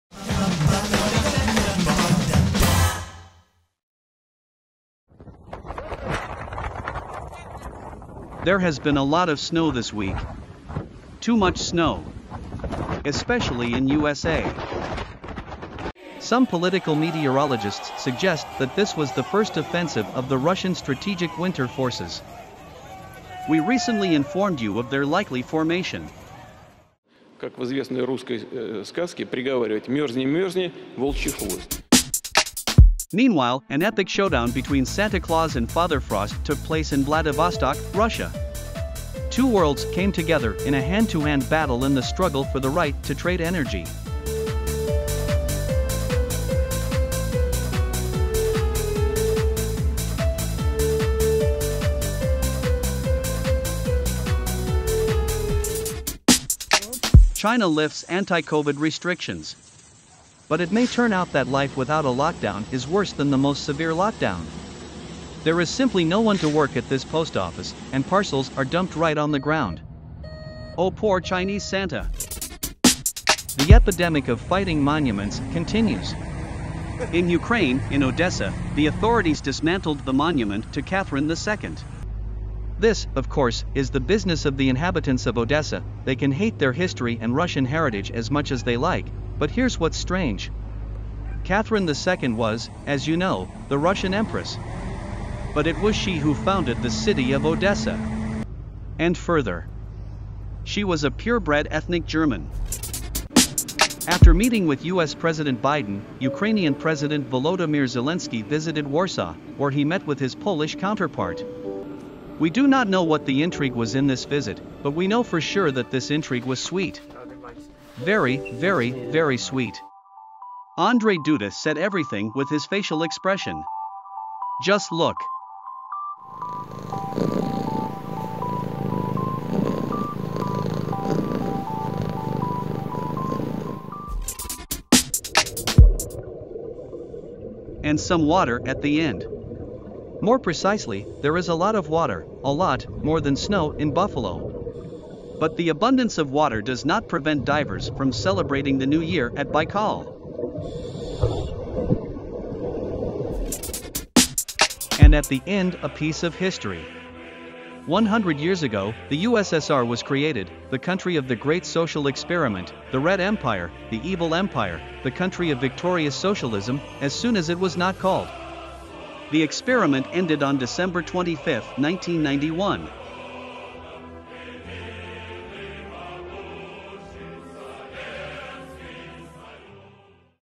Samovar News 01. Snow storm, Zelensky and Duda, and other.
Новости от Самовара. Снежная буря, Зеленский с Дудой и другое.